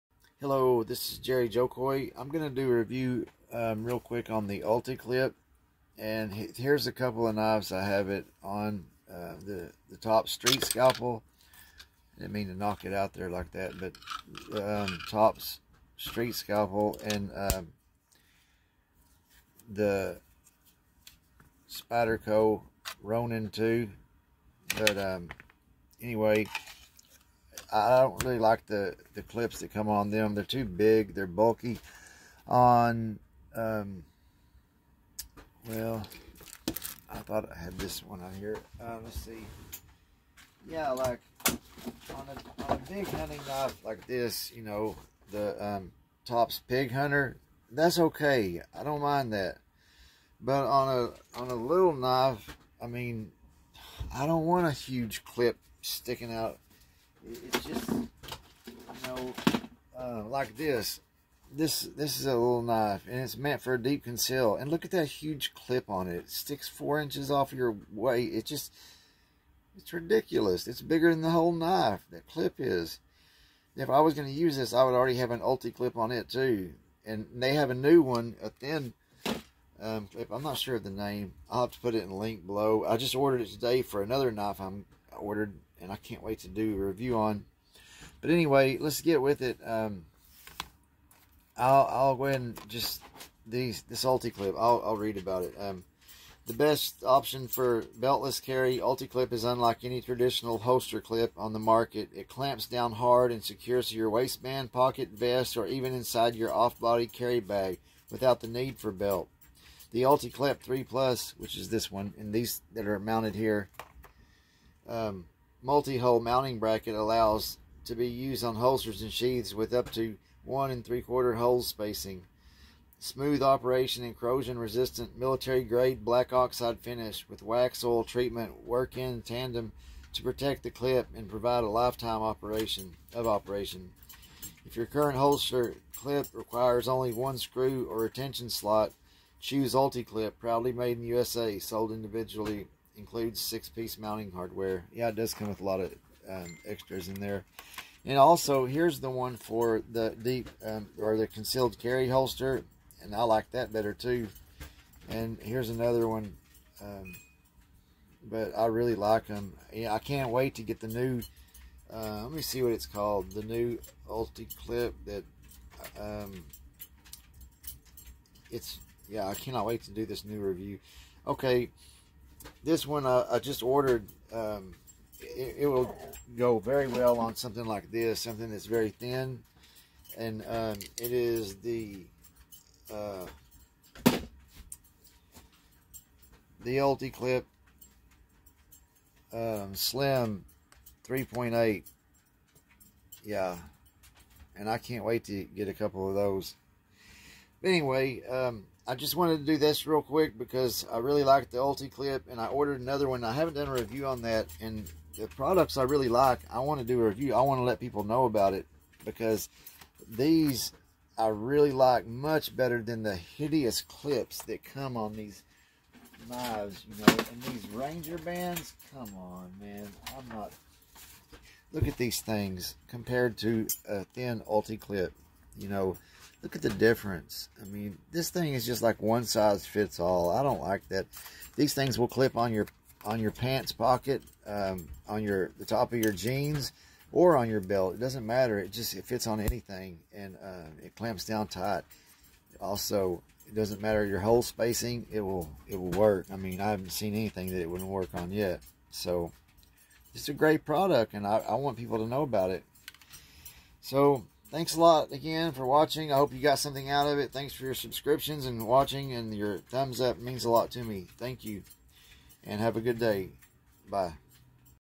ULTICLIP 3+ Great Clips for Kydex sheaths
ULTICLIP come in different sizes and styles some for concealed carry holsters others for Kydex knife sheath and the new one I ordered for a new knife that I cannot wait to get is the ULTICLIP  Slim 3.3 it is perfect smaller concealable knives I will have a review with it on my new RMJ knife as soon as I get them both in so I look forward to that thank you for watching and I appreciate your thumbs up and subscriptions have a good day.UltiClip Slim 3.3